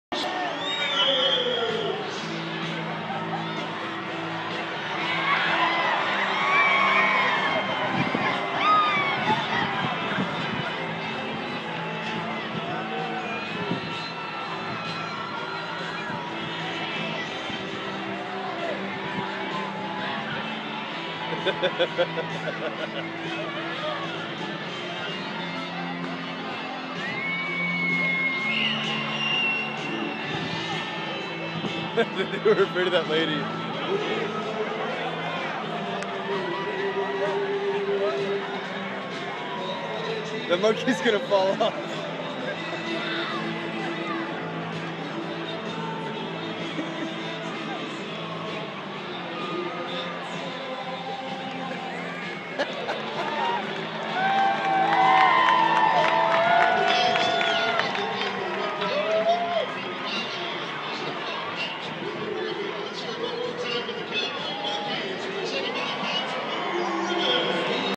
Monkey rodeo
Monkeys riding dogs herding goats at Harrisburg senators 2013